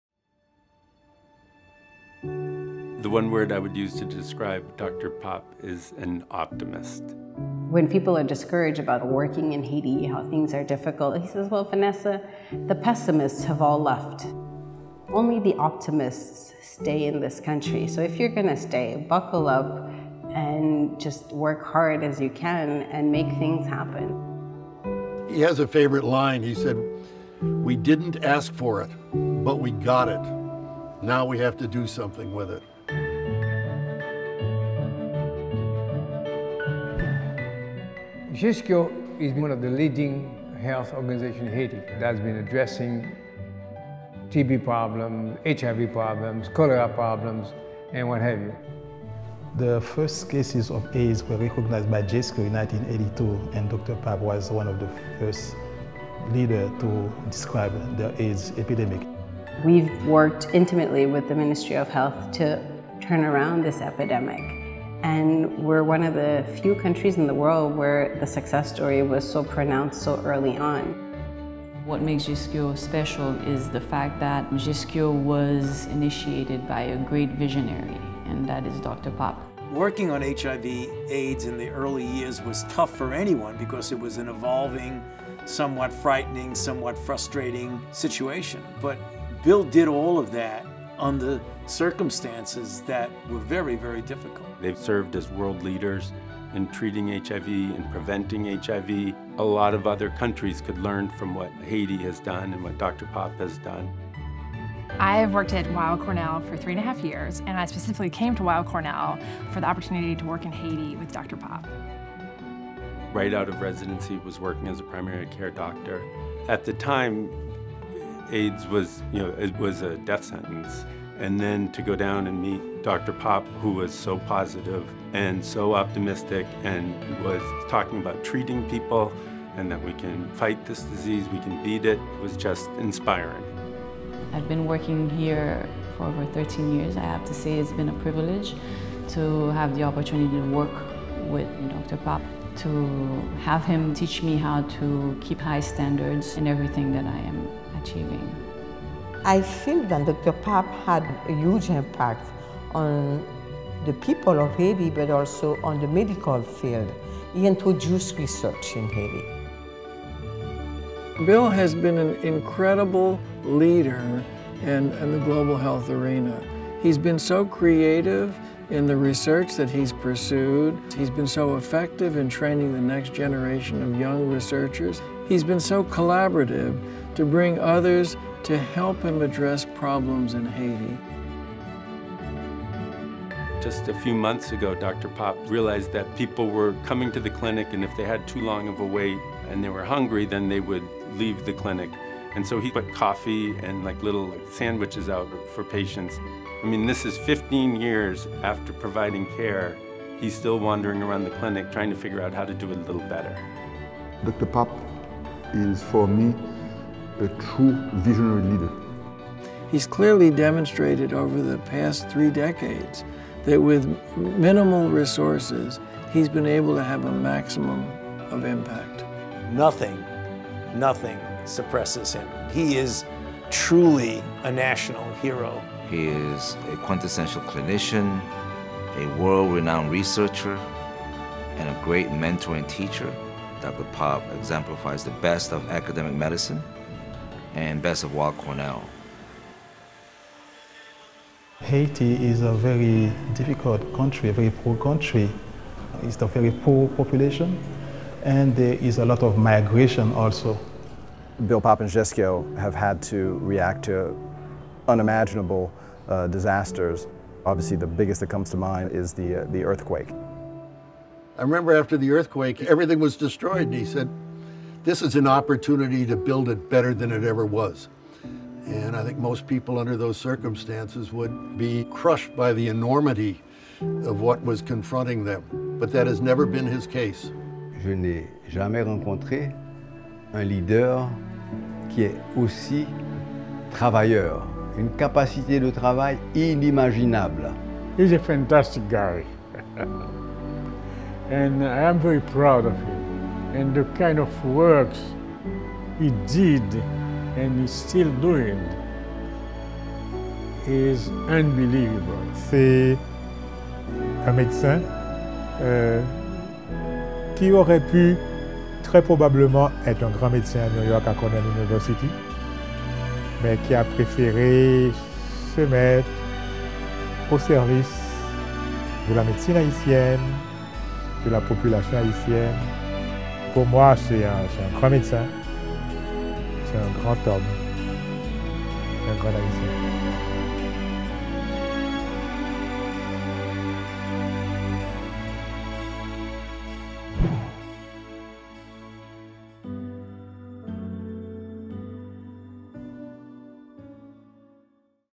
Weill Cornell Medicine Honors Dr. Jean William Pape
A pioneer in the fight against HIV/AIDS, Dr. Jean William “Bill” Pape in 1982 established GHESKIO, or the Haitian Study Group on Opportunistic Infection and Kaposi’s Sarcoma, which is believed to be the world’s oldest clinic dedicated to treating, studying and preventing the virus. Through HIV counseling, AIDS care and research into new treatment and prevention paradigms, GHESKIO has helped reduce the virus’ prevalence in Haiti from 6 percent to 2 percent since the clinic’s founding. Nearly three-quarters of Haitians infected are now on effective treatment, with survival rates comparable to those in the United States and other resource-rich countries.

Dr. Pape, the Howard and Carol Holtzmann Professor in Clinical Medicine and the founder and director of GHESKIO in Haiti, has been awarded the inaugural Joan and Sanford I. Weill Exemplary Achievement Award from Weill Cornell Medicine.